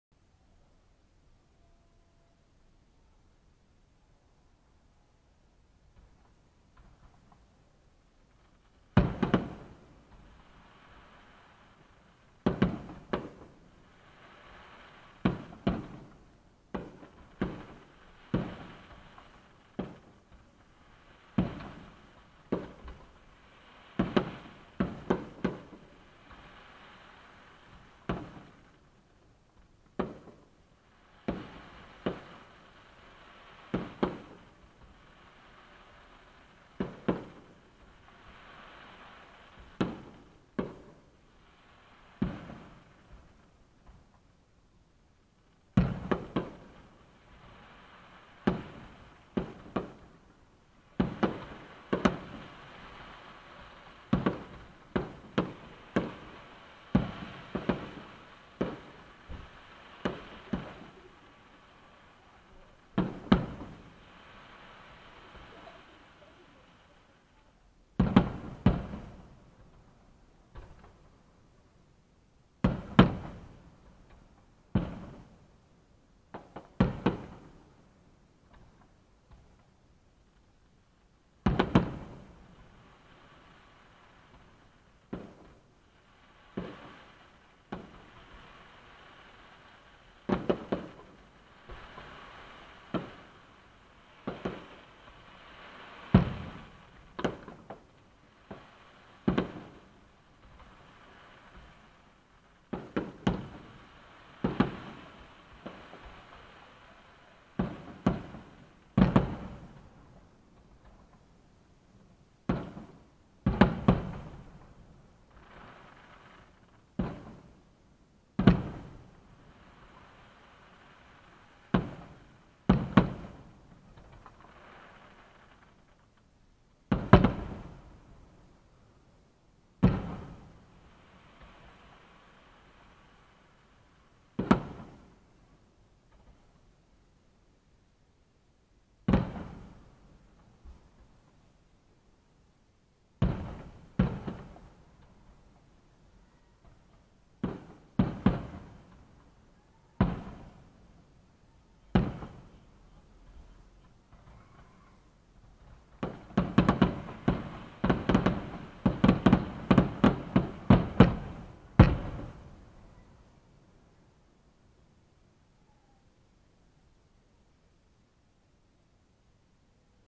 Melton Mowbray Christmas Light Switch on 2018
Couldn't record due to having my son on my shoulders, but then found the car cam had picked it up.